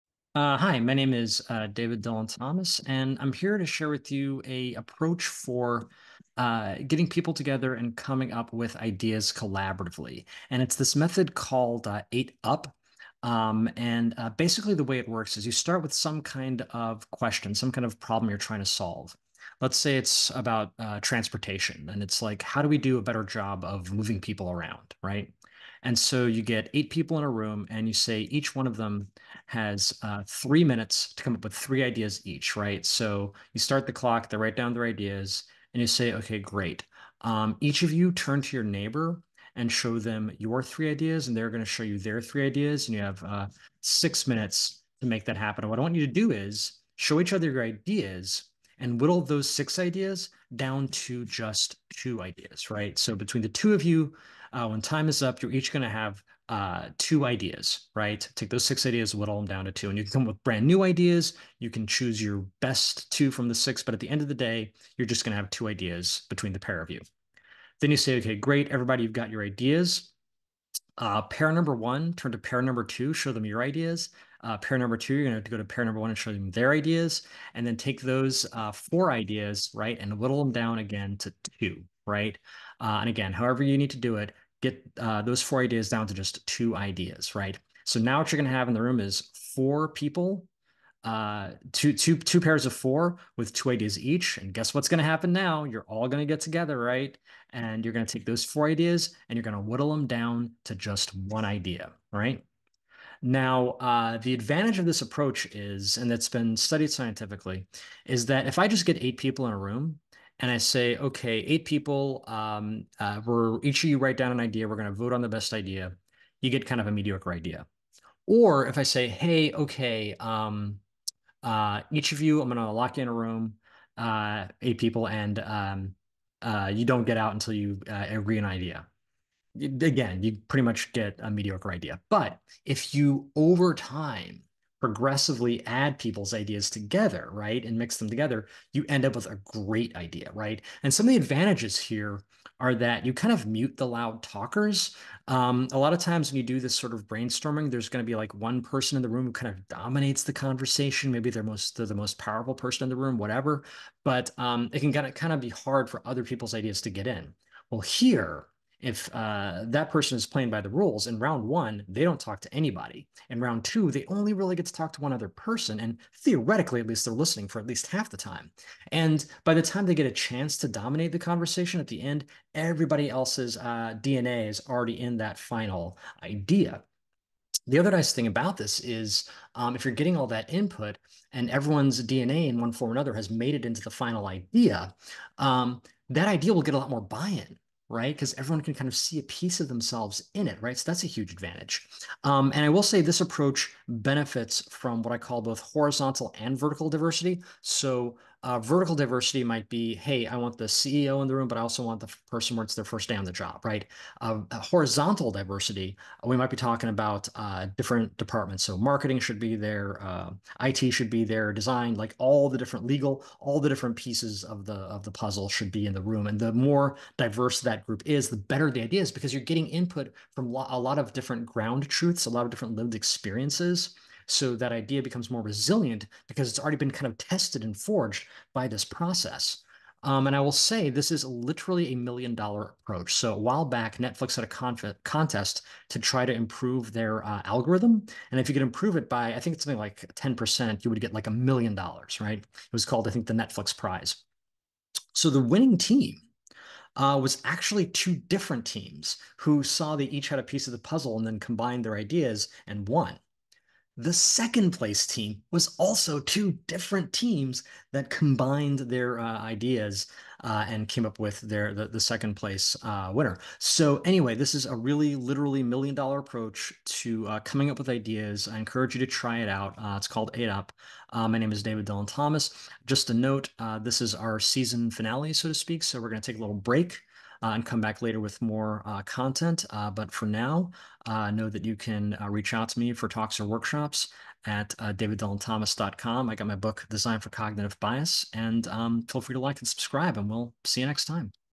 How to collaborate on an idea and make sure everyone gets heard
The 8-up method for coming up with ideas is a literally million dollar approach that rewards diversity and produces resilient, innovative ideas that get buy-in. (And also mutes the loud talkers who tend to dominate conversations).

Learn more about the 2006 Netflix Prize winner and how they combined two ideas to make $1 million

Learn more about David Dylan Thomas at daviddylanthomas.com

Read a sample chapter of Dave's book "Design for Cognitive Bias"